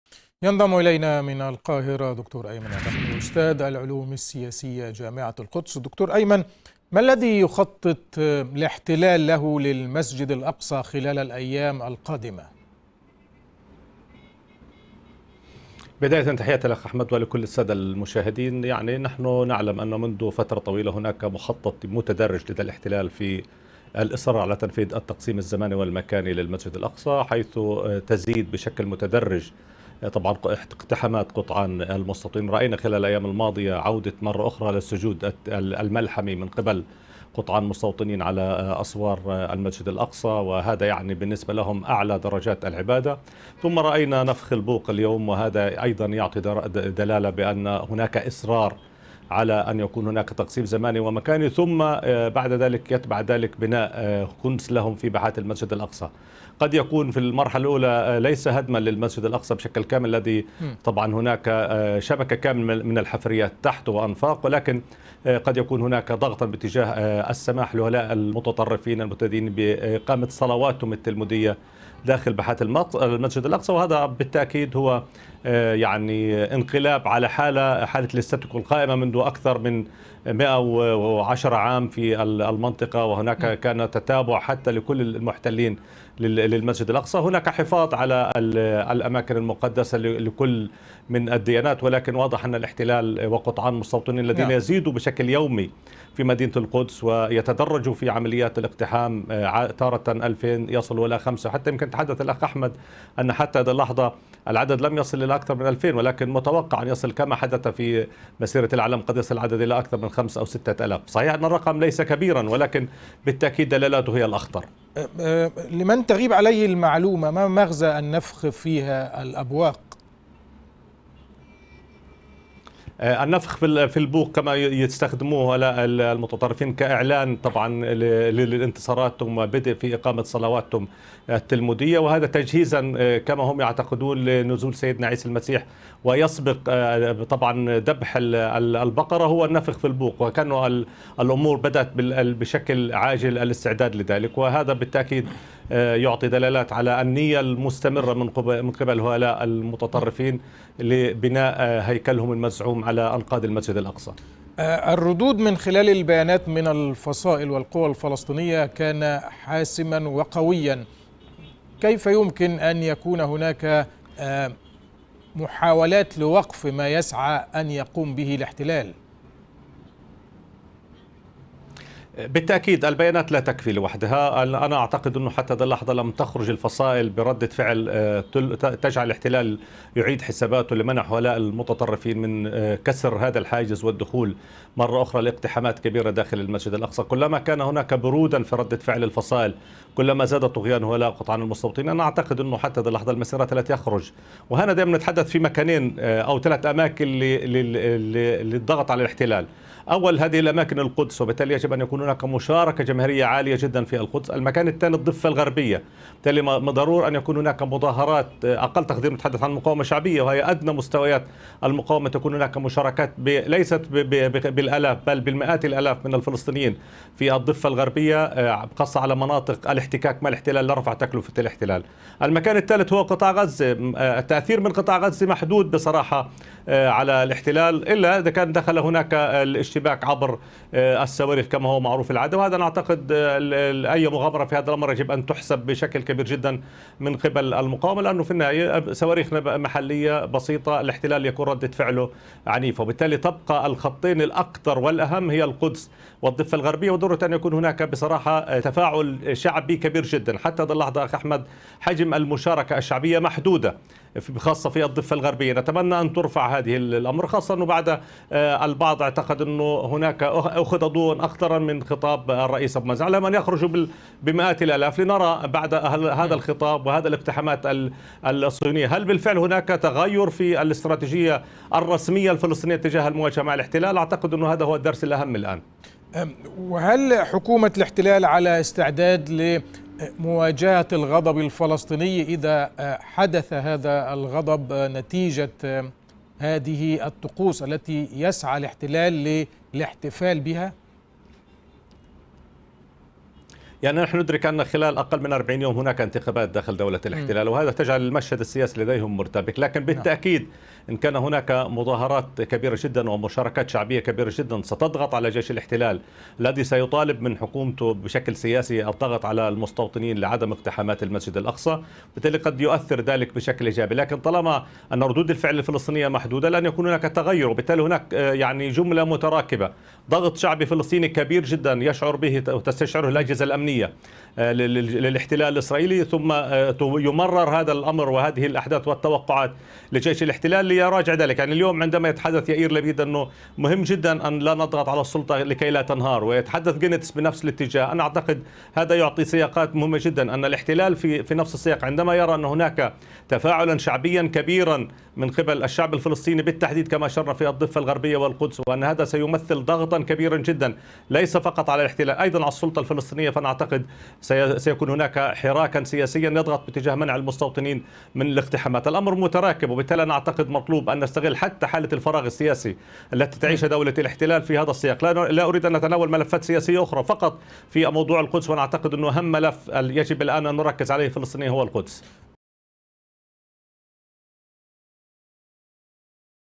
الأيام المقبلة.. ماذا يخطط الاحتلال الإسرائيلي للمسجد الأقصى؟
دعت الفصائل_الفلسطينية أهالي الضفة الغربية والقدس المحتلة لـ النفير وحماية المسجد الأقصى من الاقتحامات المتواصلة من قبل المستوطنين واعتبرت الاقتحامات وممارسة الطقوس الاستفزازية تصعيدا خطيرا لا يمكن السكوت عليه.
تفاصيل أكثر عن هذا الموضوع مع ضيف قناة الغد من القاهرة، أستاذ العلوم السياسية في جامعة القدس، الدكتور أيمن الرقب.